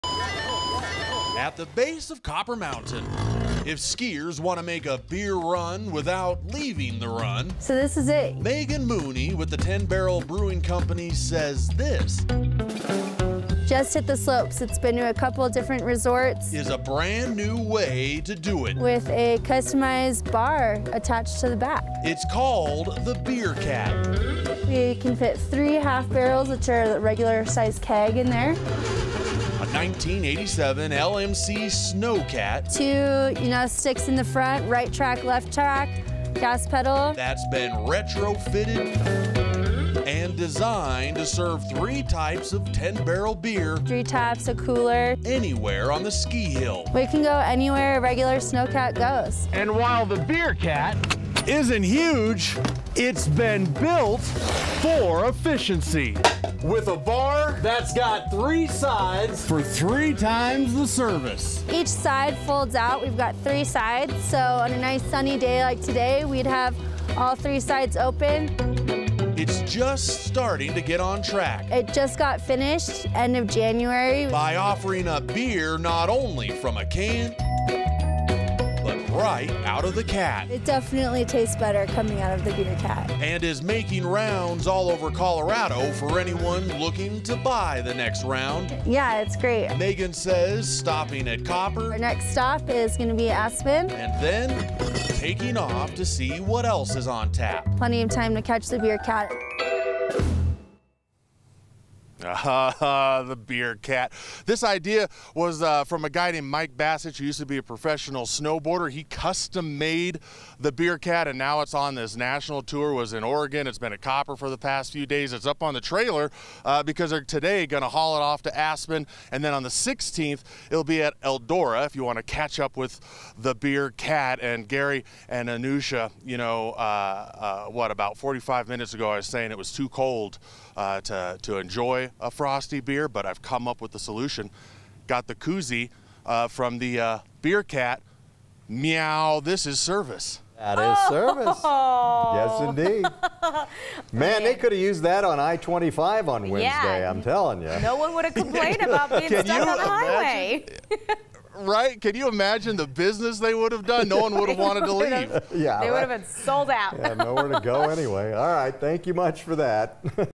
Beer cat roams around different Colorado ski areas
The Beer Cat has been to several ski areas already and will pop at several more over the next few weeks.

9NEWS (KUSA) is located in Denver, Colorado.